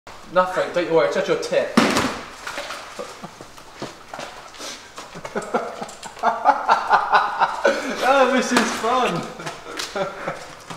MrWam & Wamgame Restaurant Scene teaser 2
Paddy & Louis get their revenge on Aiden the waiter